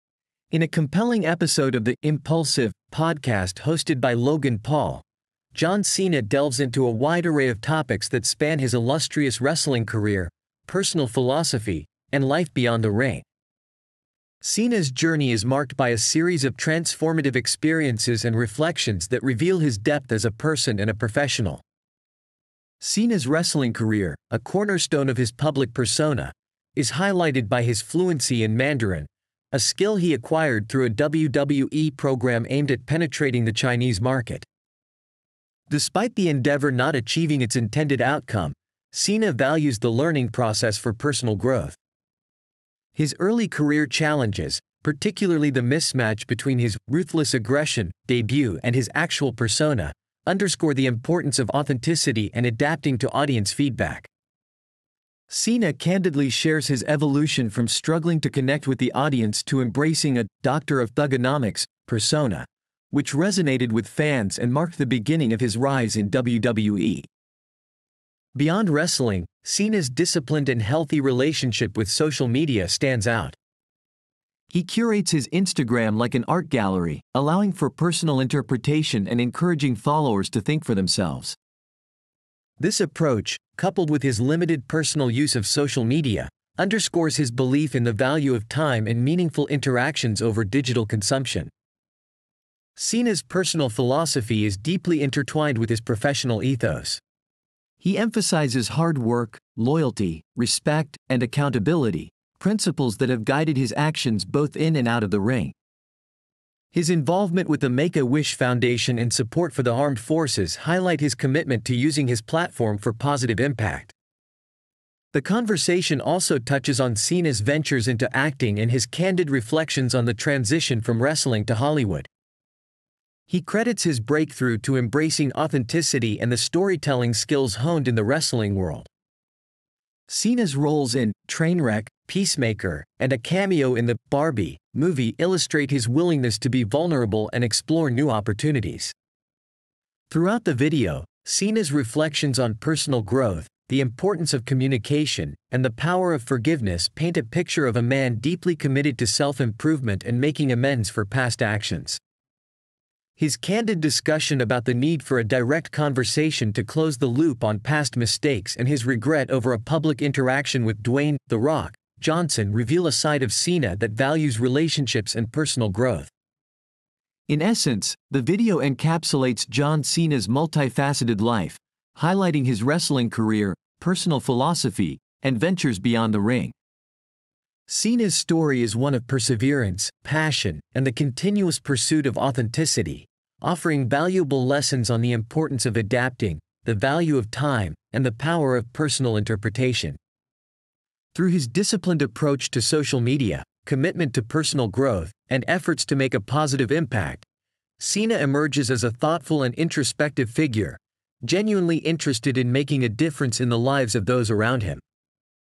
IMPAULSIVE: THE JOHN CENA INTERVİEW - IMPAULSIVE EP. 407: VİDEO SUMMARY
In a compelling episode of the "Impulsive" podcast hosted by Logan Paul, John Cena delves into a wide array of topics that span his illustrious wrestling career, personal philosophy, and life beyond the ring. Cena's journey is marked by a series of transformative experiences and reflections that reveal his depth as a person and a professional.

Cena's wrestling career, a cornerstone of his public persona, is highlighted by his fluency in Mandarin, a skill he acquired through a WWE program aimed at penetrating the Chinese market. Despite the endeavor not achieving its intended outcome, Cena values the learning process for personal growth. His early career challenges, particularly the mismatch between his "ruthless aggression" debut and his actual persona, underscore the importance of authenticity and adapting to audience feedback. Cena candidly shares his evolution from struggling to connect with the audience to embracing a "Doctor of Thuganomics" persona, which resonated with fans and marked the beginning of his rise in WWE.

Beyond wrestling, Cena's disciplined and healthy relationship with social media stands out. He curates his Instagram like an art gallery, allowing for personal interpretation and encouraging followers to think for themselves. This approach, coupled with his limited personal use of social media, underscores his belief in the value of time and meaningful interactions over digital consumption.

Cena's personal philosophy is deeply intertwined with his professional ethos. He emphasizes hard work, loyalty, respect, and accountability, principles that have guided his actions both in and out of the ring. His involvement with the Make-A-Wish Foundation and support for the armed forces highlight his commitment to using his platform for positive impact.

The conversation also touches on Cena's ventures into acting and his candid reflections on the transition from wrestling to Hollywood. He credits his breakthrough to embracing authenticity and the storytelling skills honed in the wrestling world. Cena's roles in "Trainwreck," "Peacemaker," and a cameo in the "Barbie" movie illustrate his willingness to be vulnerable and explore new opportunities.

Throughout the video, Cena's reflections on personal growth, the importance of communication, and the power of forgiveness paint a picture of a man deeply committed to self-improvement and making amends for past actions. His candid discussion about the need for a direct conversation to close the loop on past mistakes and his regret over a public interaction with Dwayne "The Rock" Johnson reveal a side of Cena that values relationships and personal growth.

In essence, the video encapsulates John Cena's multifaceted life, highlighting his wrestling career, personal philosophy, and ventures beyond the ring. Cena's story is one of perseverance, passion, and the continuous pursuit of authenticity, offering valuable lessons on the importance of adapting, the value of time, and the power of personal interpretation. Through his disciplined approach to social media, commitment to personal growth, and efforts to make a positive impact, Cena emerges as a thoughtful and introspective figure, genuinely interested in making a difference in the lives of those around him.